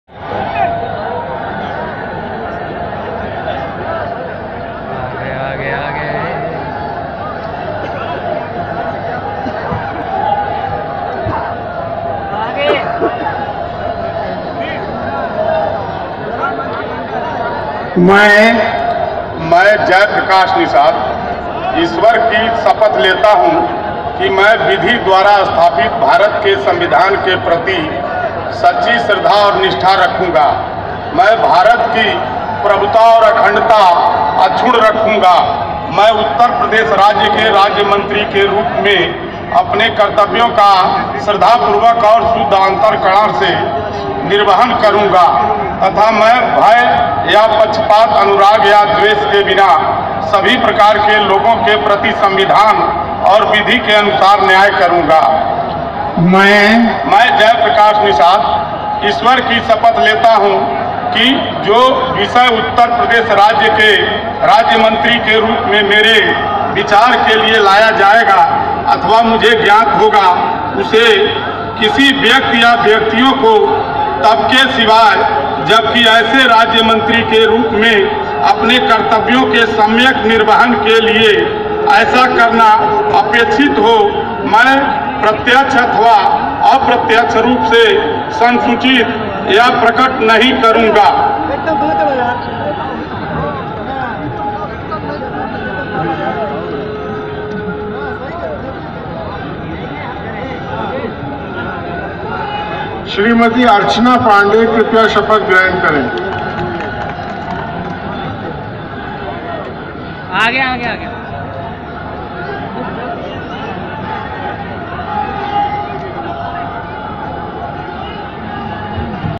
SH. JAI PRAKASH NISHAD JI
Sh. Jai prakash Nishad ji oath ceremony as a STATE MINISTER UTTAR PRADESH GOVT.